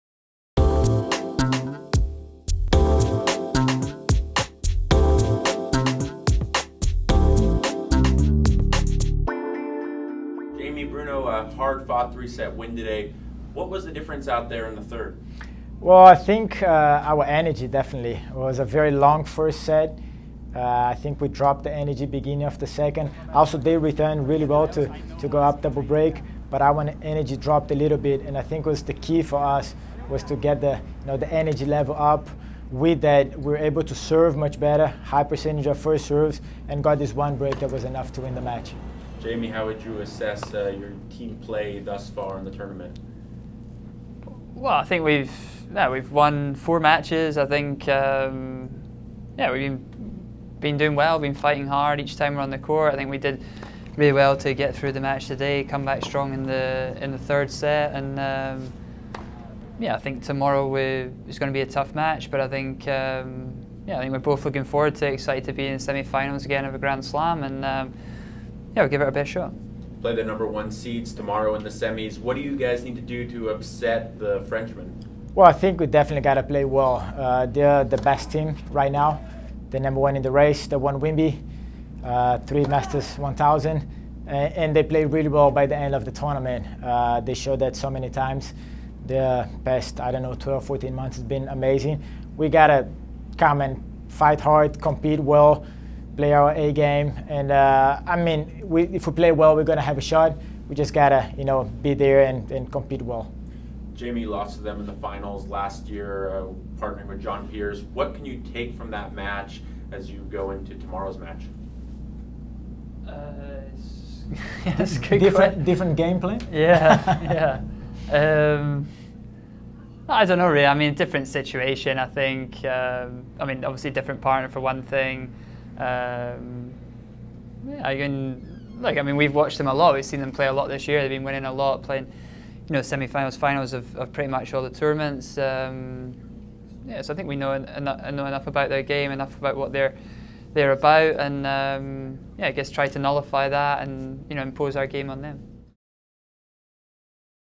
Murray Soares Get Ready For US Open 2016 SFs
Jamie Murray and Bruno Soares assess their game as they get ready to face defending champions Pierre-Hugues Herbert and Nicolas Mahut in the US Open semi-finals.